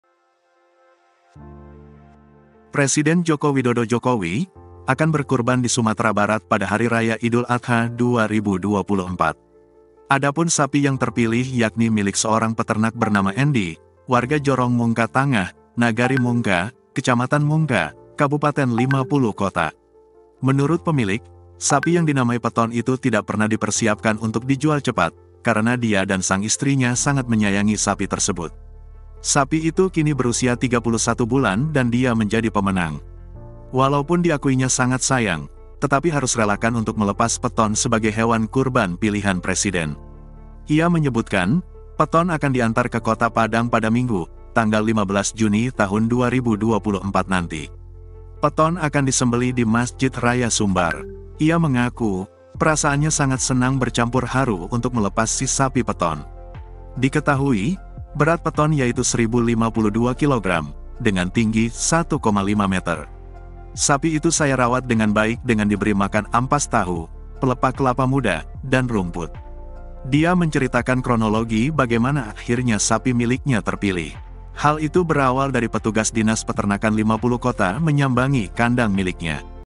Penampakan Sapi 1 Ton, Pilihan Jokowi untuk Kurban di Sumbar
Presiden Joko Widodo Jokowi, akan berkurban di Sumatera Barat pada Hari Raya Idul Adha 2024.

Adapun sapi yang terpilih yakni milik seorang peternak bernama Endi, warga Jorong Mungka Tangah, Nagari Mungka, Kecamatan Mungka, Kabupaten Limapuluh Kota.

video editor HR Hendro sandi